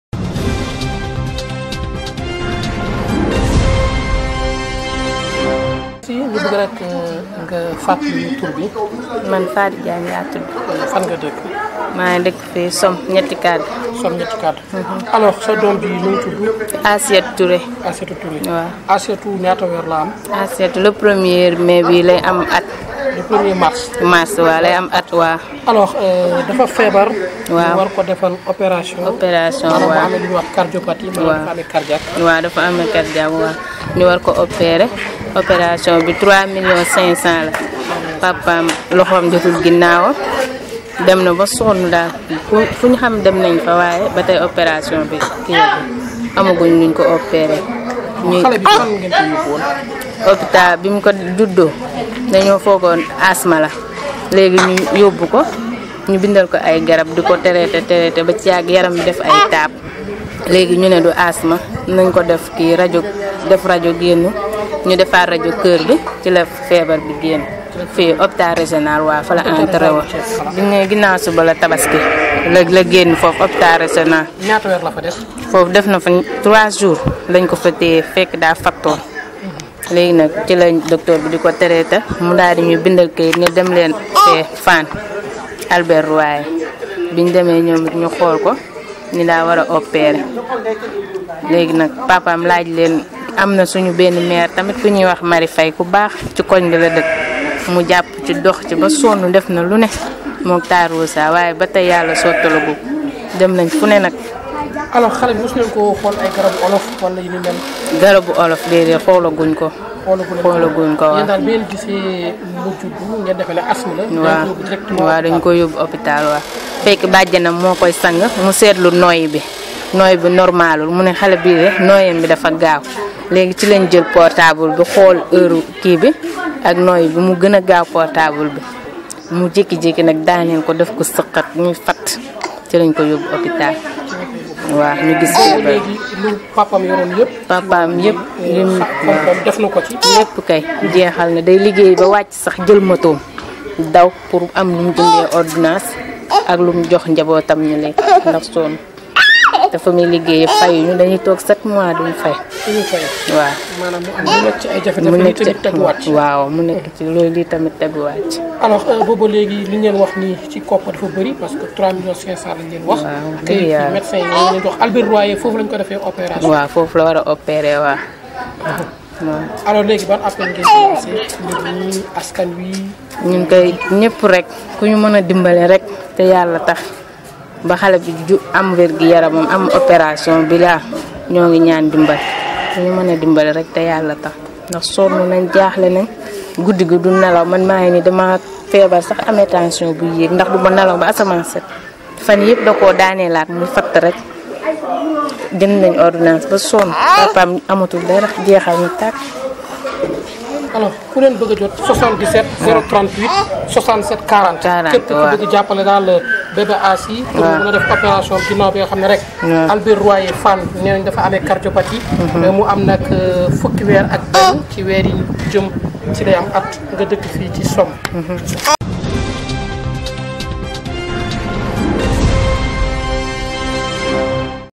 Fatou Diallo Niéty Kade Le 22 Février 2018 1
cet enfant est atteinte d'une maladie grave et doit être opéré d'urgence